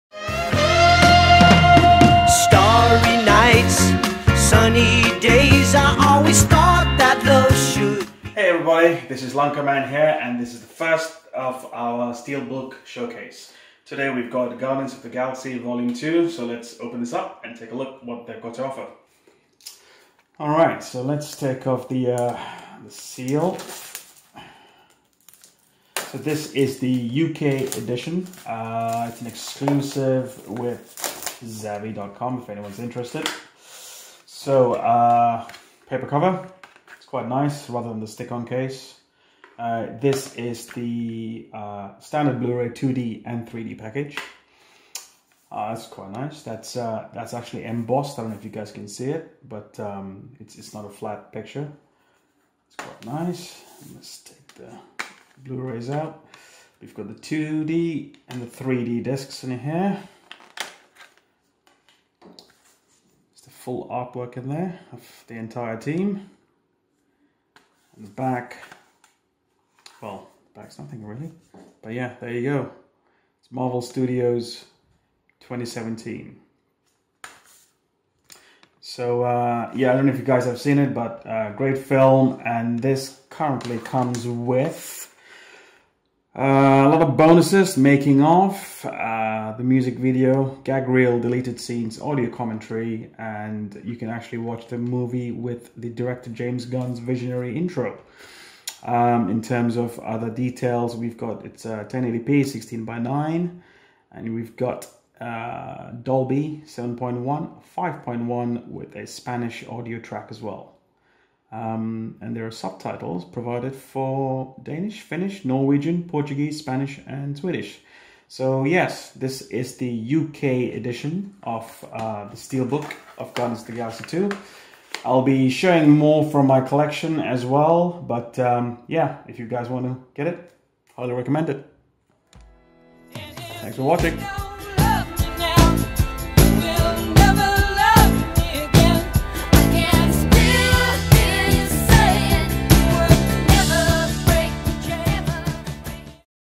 Guardians of the Galaxy Volume 2 STEELBOOK || LANKA MAN || BLU RAY
A quick overview of the UK BLU RAY STEELBOOK edition of the movie GUARDIANS OF THE GALAXY VOL 2.

Filmed on the Sony Xperia XZ phone (by Arundhathy)